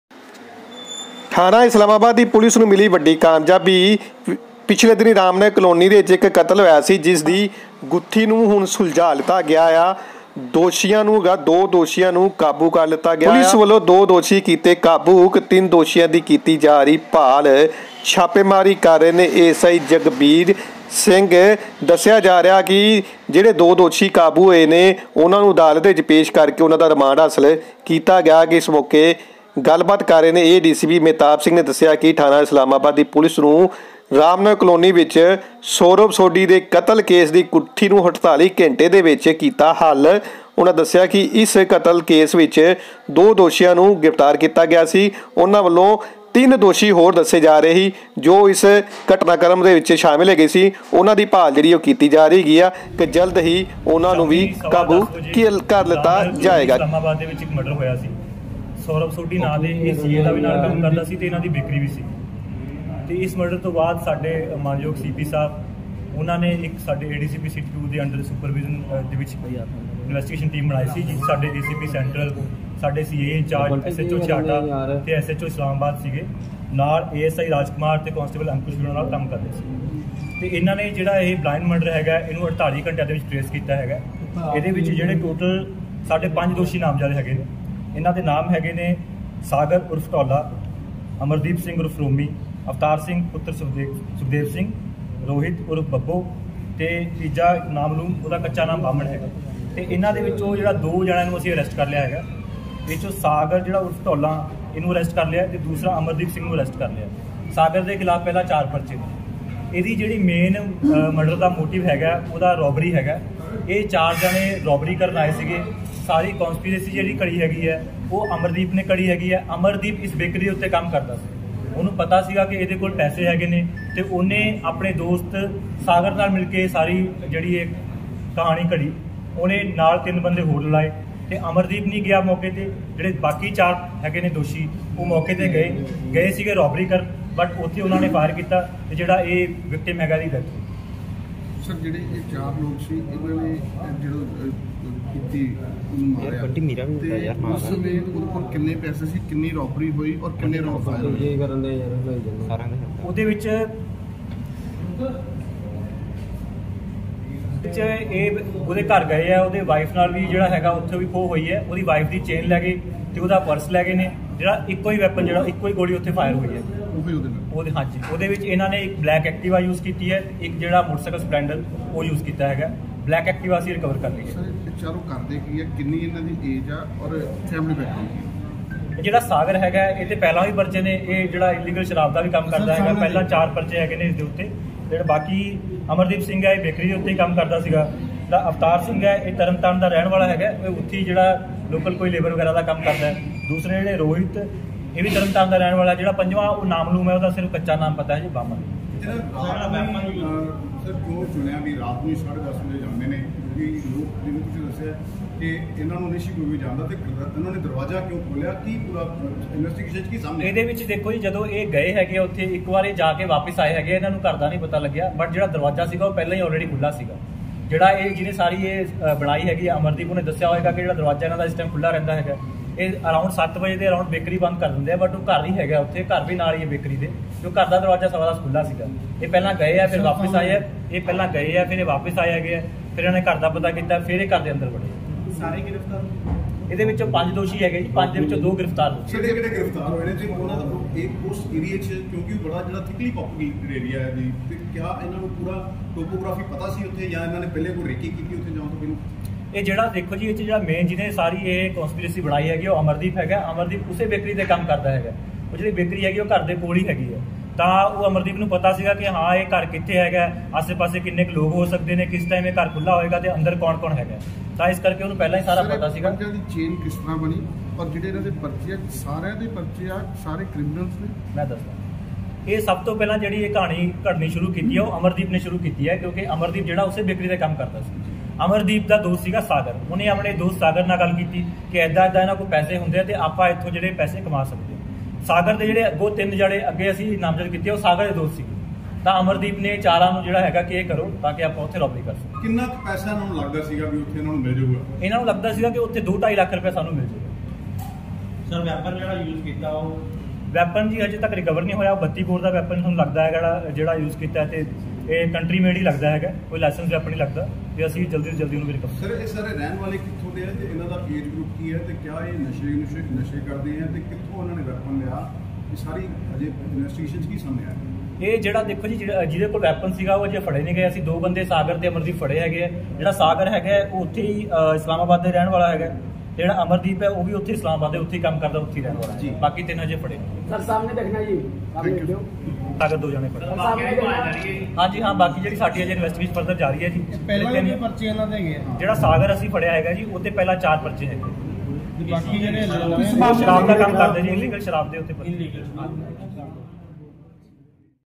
Amritsar ਪੁਲਿਸ ਨੇ ਅੰਨ੍ਹੇ ਕਤਲ ਦੀ ਗੁੱਥੀ 24 ਘੰਟੇ ਵਿਚ ਸੁਲਜ਼ਾਈ 2 ਮੁਜ਼ਰਿਮ ਕਾਬੂ 3 ਦੀ ਭਾਲ
Amritsar ਪੁਲਿਸ ਨੇ ਅੰਨ੍ਹੇ ਕਤਲ ਦੀ ਗੁੱਥੀ 24 ਘੰਟੇ ਵਿਚ ਸੁਲਜ਼ਾਈ 2 ਮੁਜ਼ਰਿਮ ਕਾਬੂ 3 ਦੀ ਭਾਲ ਜਾਰ